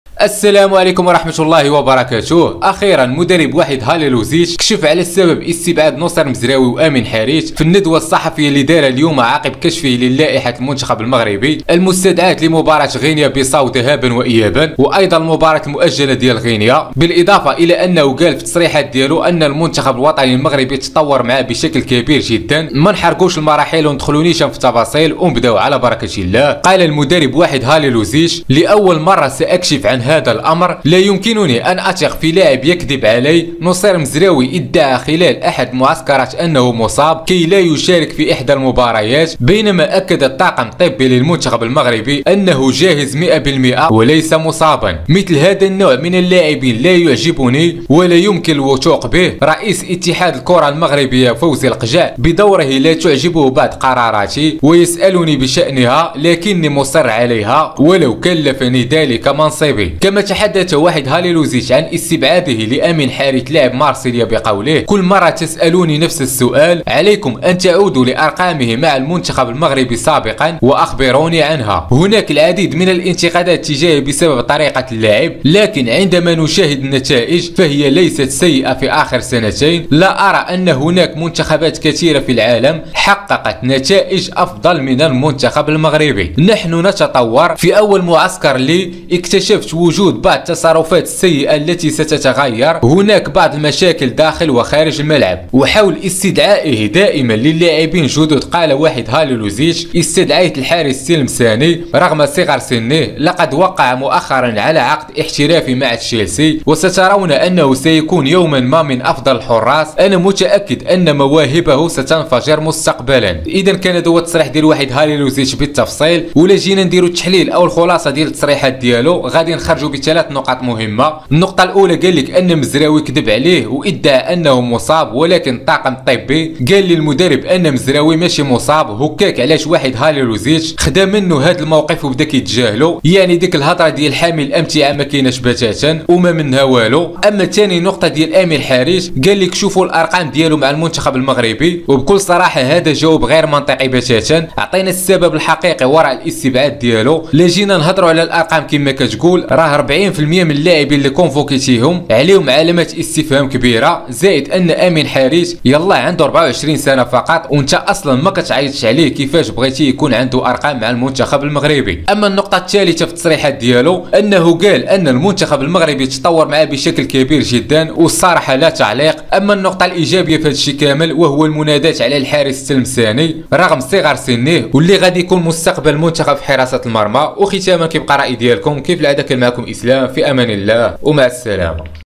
عاااجل😱وحيد خليلوزيتش يكشف السر وراء استبعاد نصير مزراوي و امين حارت من لائحة المنتخب المغربي
عاااجل وحيد خليلوزيتش يكشف السر وراء استبعاد نصير مزراوي و امين حارت من لائحة المنتخب المغربي غينيا بيساو
حراسة المرمى: ياسين بونو- أنس الزنيتي- منير المحمدي- سامي التلمساني

الدفاع: أشرف حكيمي- آدم ماسينا- أيوب العملود- سفيان الكرواني- سامي مايي- سفيان شاكله- غانم سايس- نايف أكرد

الوسط: فيصل فجر- أيمن برقوق- سفيان أمرابط- عمران لوزا- إلياس الشعير- سليم أملاح، عبدو هروي

الهجوم: أشرف بنشرقي- منير الحدادي- سفيان بوفال- سفيان رحيمي- يوسف النصيري- أيوب الكعبي- زكرياء بوخلال- ريان مايي